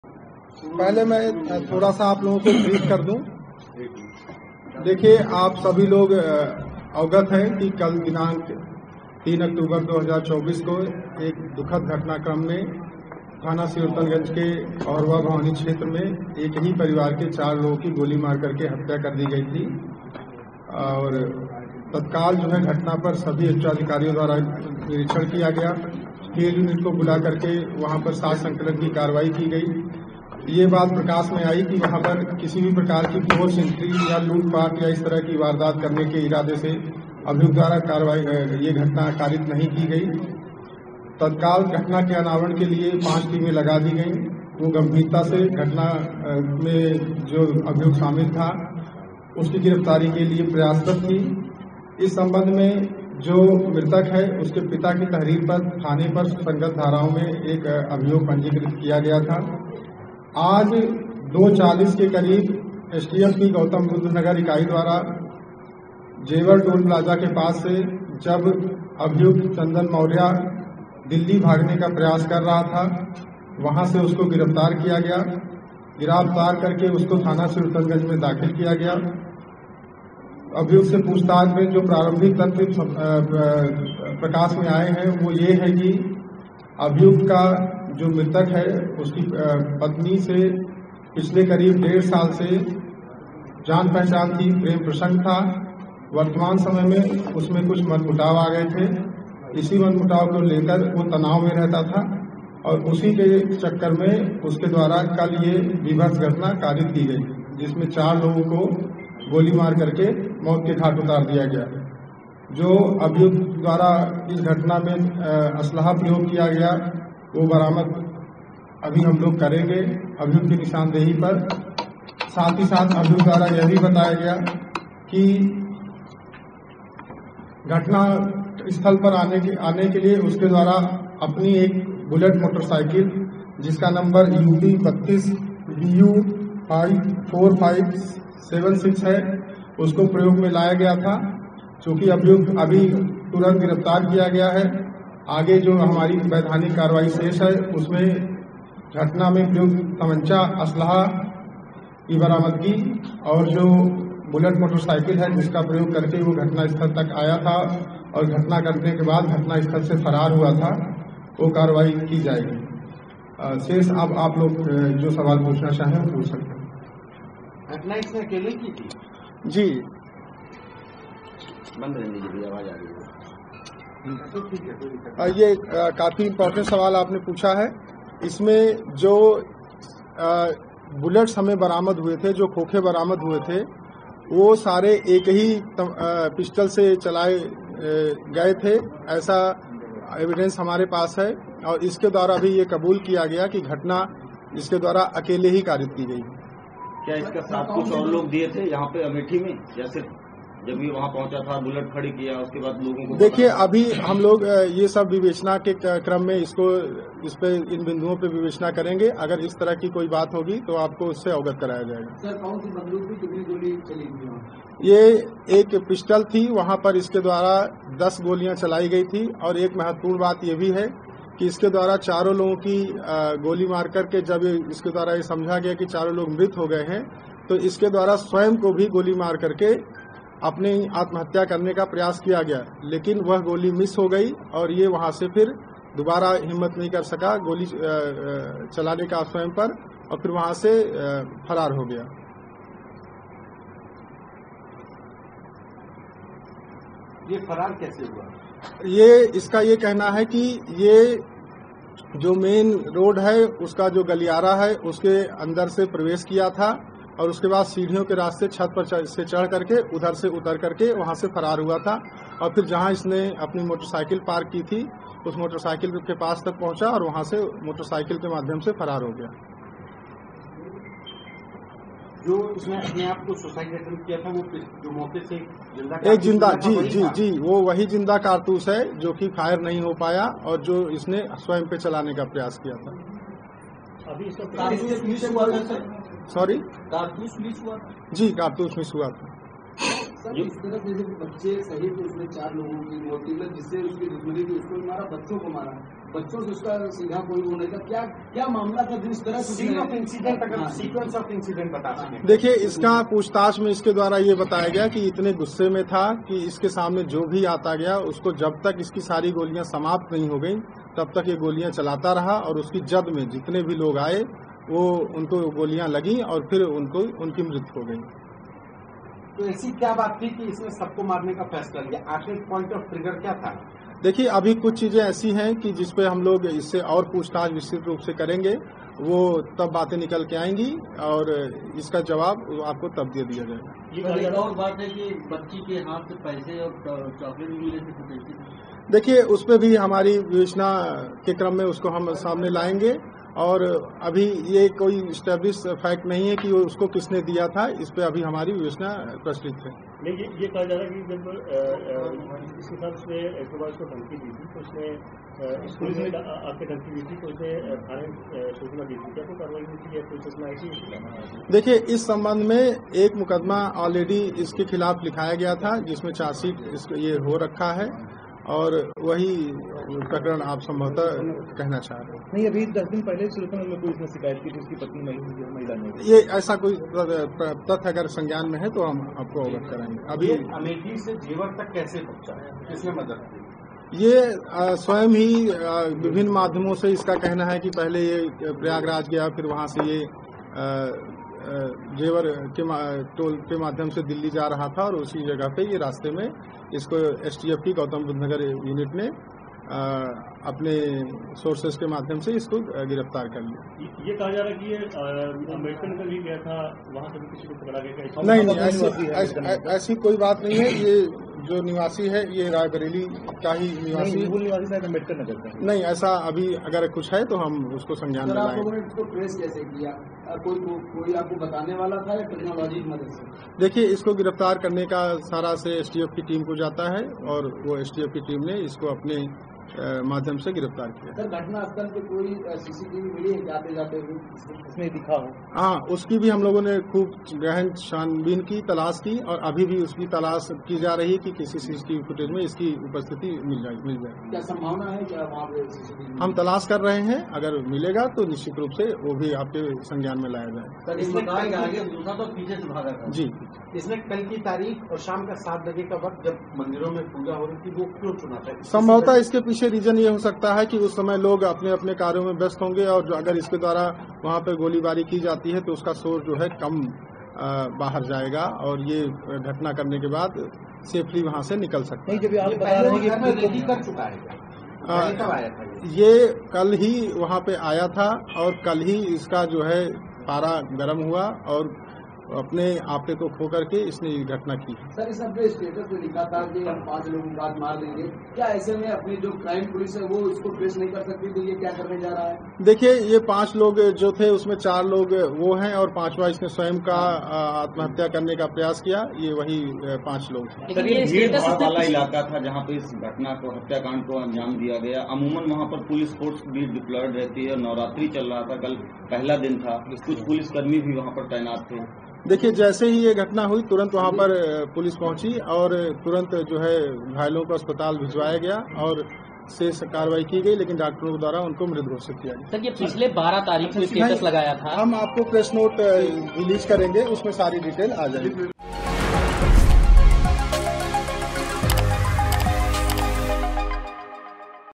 Amethi Family Kand: Teacher की पत्नी से संबंध, अमेठी कांड के आरोपी पर पुलिस का खुलासा | NBT
अमेठी कांड में 4 लोगों की जान लेने वाला चंदन वर्मा शनिवार को पुलिस के हत्थे चढ़ गया । पुलिस ने शनिवार शाम उसे धर दबोचा । इसके बाद पुलिस ने पूरे मामले का खुलासा करते हुए प्रेस कॉन्फ्रेंस में हर सवाल के जवाब दिया । पुलिस ने दावा किया कि आरोपी ने वारदात को अंजाम देने के बाद खुद को भी मारने की कोशिश कर रहा था लेकिन गन नहीं चली। साथ ही आरोपी और मृतक की पत्नी का डेढ़ साल से प्रेम प्रसंग था ।